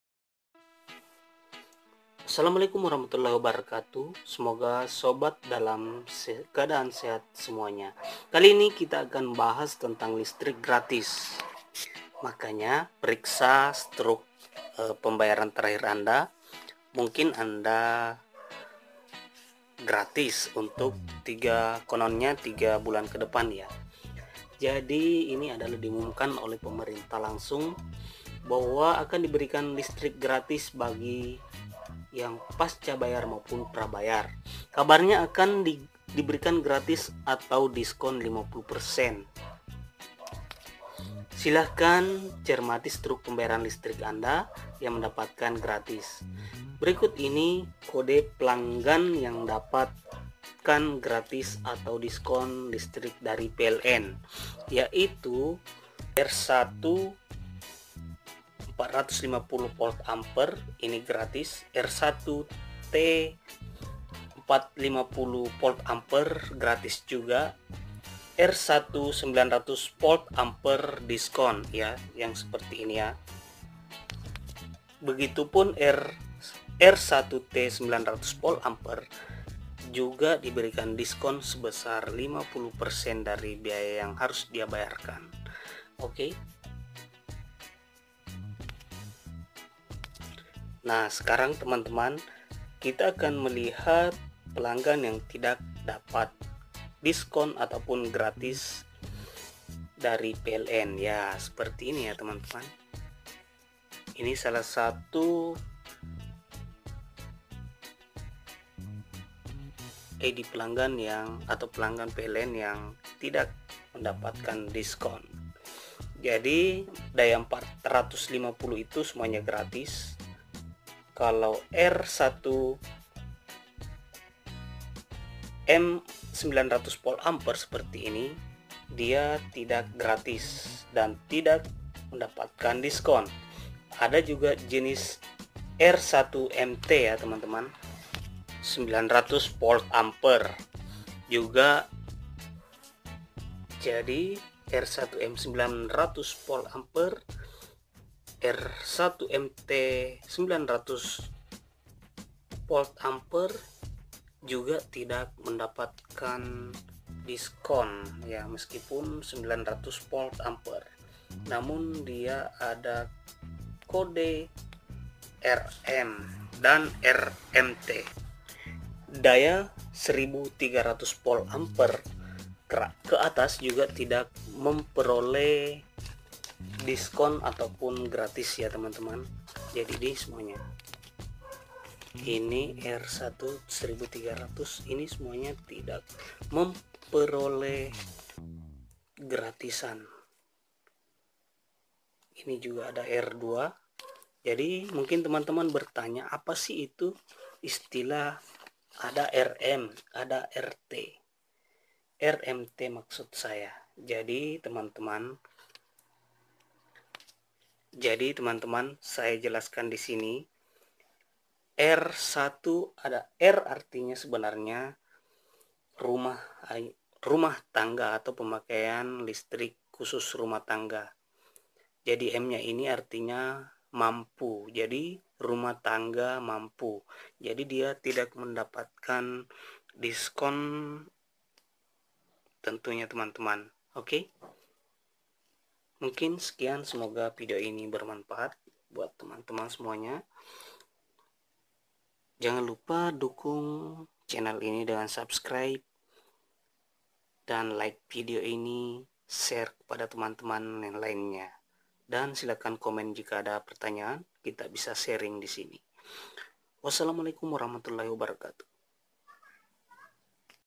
Cara Mengetahui Apakah Anda Dapat Listrik Gratis 3 Bulan Dari Pemerintah
Dalam video ini diberikan informasi bagaimana cara mengetahui bahwa anda dapat listrik gratis/diskon dari pemerintah dan juga kami berikan siapa saja yang tidak dapat apa-apa. Hal ini diberikan dari pemrintah akibat dari efek dari C0vid 19.